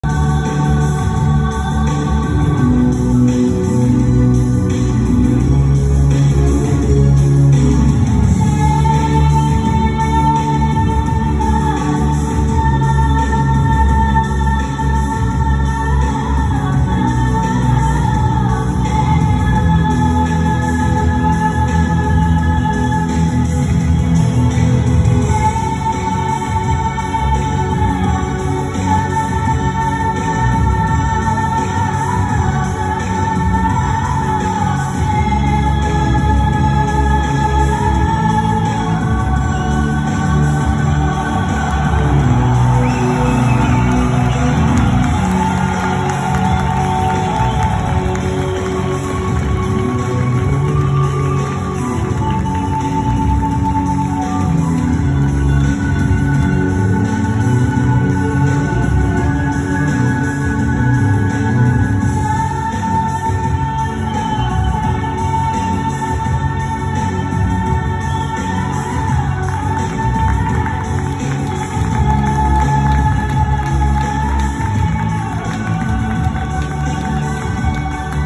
Tervel Pulev Entrance vs Deshon Webster! Plovdiv,Bulgaria (14.12.2019)
Tervel Pulev vs Deshon Webster for the international Title!